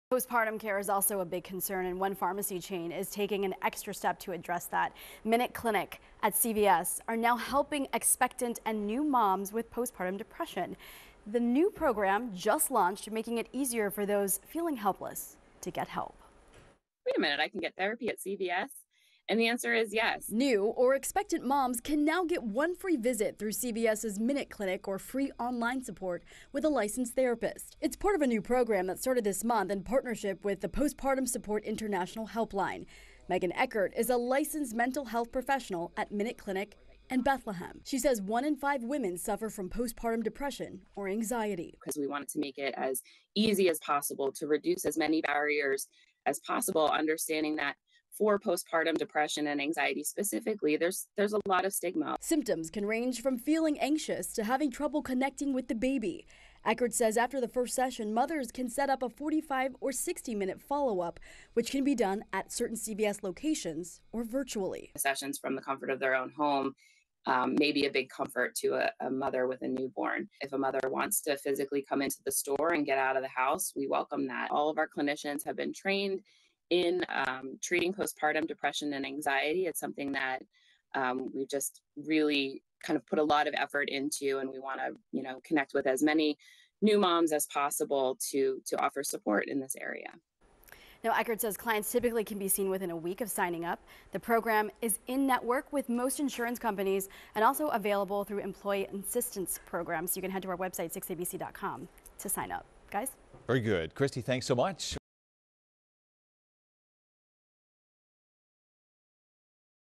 CVS MinuteClinic launches new program to help expectant, new moms battling postpartum depression
Postpartum care is a big concern for new mothers and one pharmacy chain is taking an extra step to address it.

MinuteClinics at CVS are now helping expectant and new moms with postpartum depression.